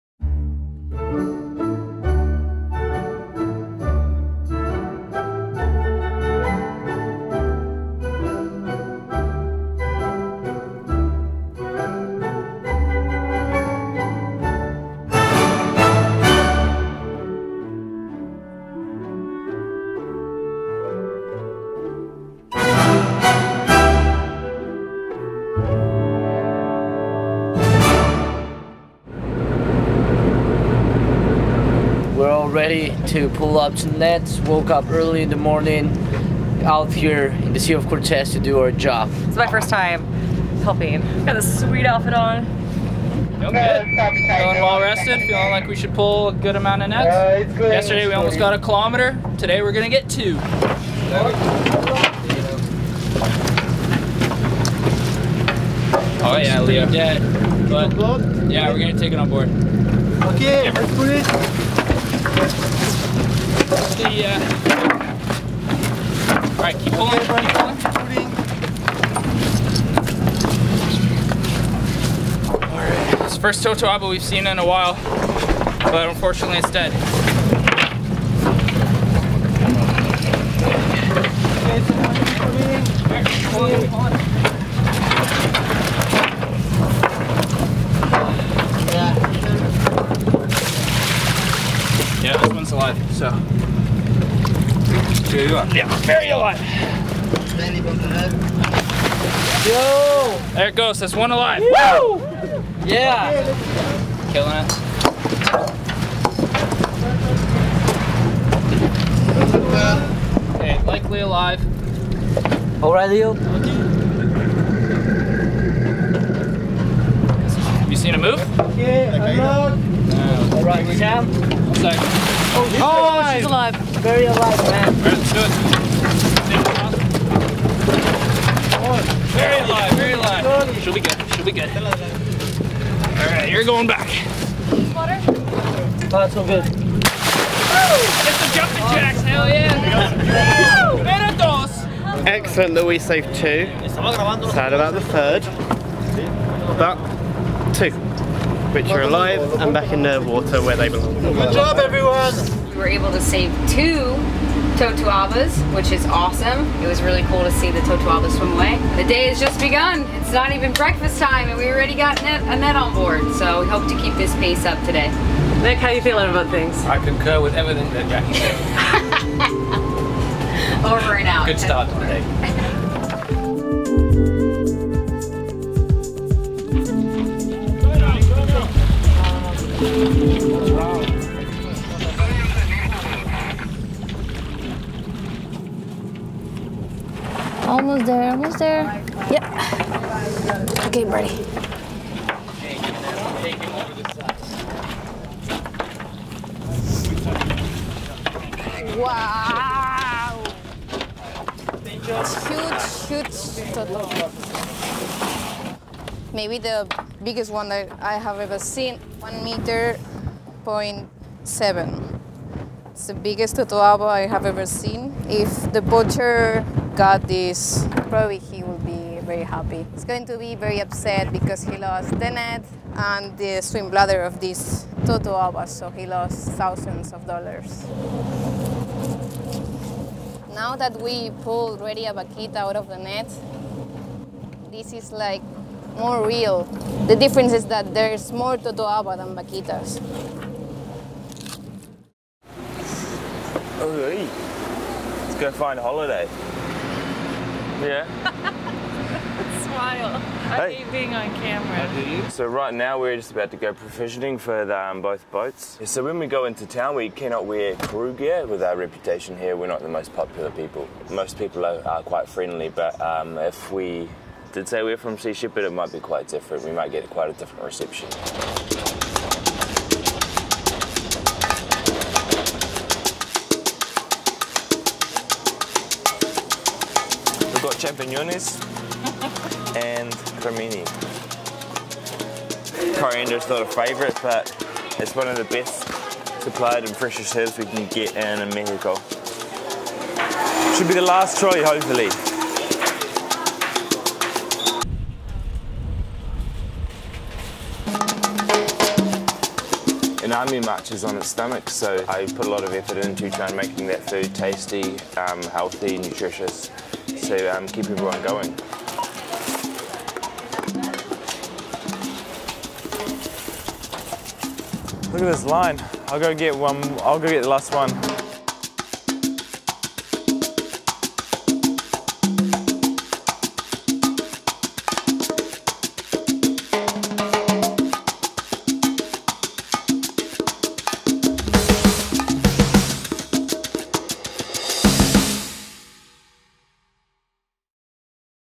Op Milagro V Episode 4 - TWO SHIPS ONE TEAM
OpMilagro is Sea Shepherd's campaign to protect the most endangered marine mammal in the world - Mexico's Vaquita porpoise.